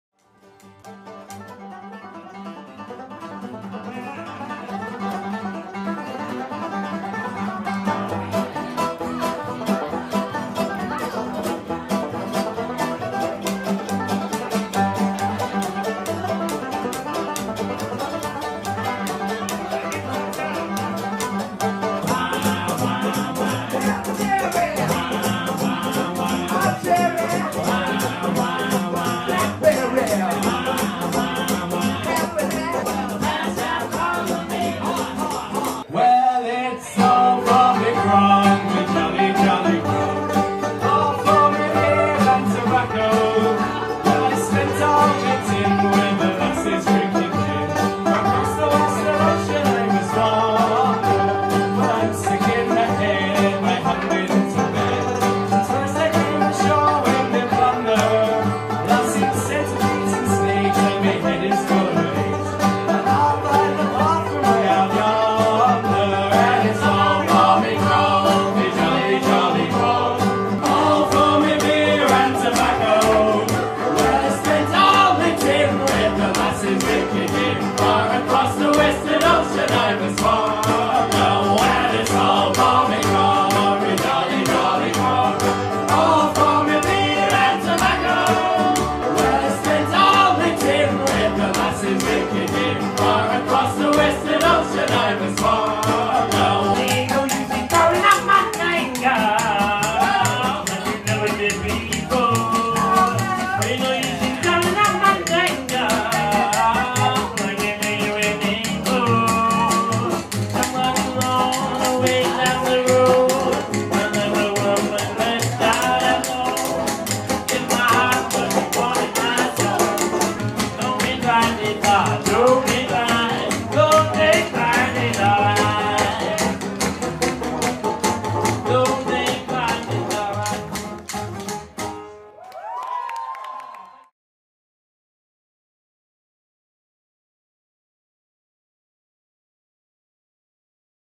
Hire The Bluegrass Tommys Band To Hire From Knees Up Agency
They have an extensive repertoire of classic country, bluegrass, Irish jigs, reels and work songs. The band provides an ideal backdrop to any event or party. Their music is perfect for dancing, dining or enjoying a pint of real ale at countryside wedding.